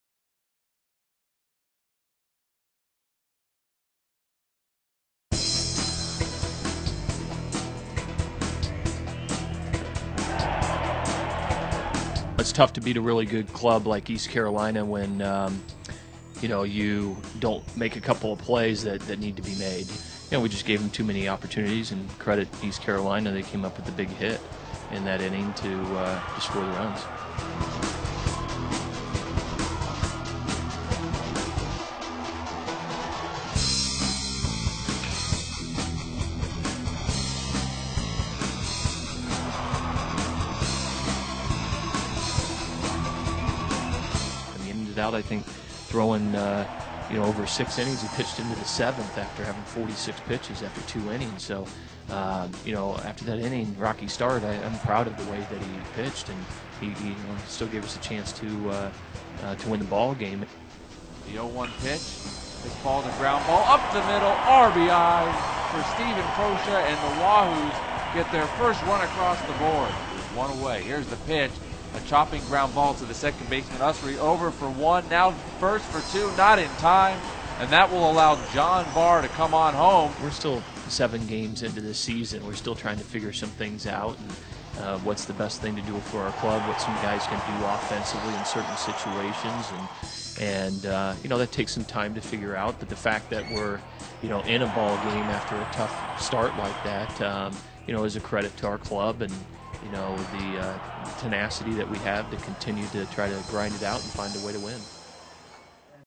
Baseball Highlights - East Carolina
Four-Run Second Inning Lifts ECU to 4-3 Win over No. 10 Cavaliers.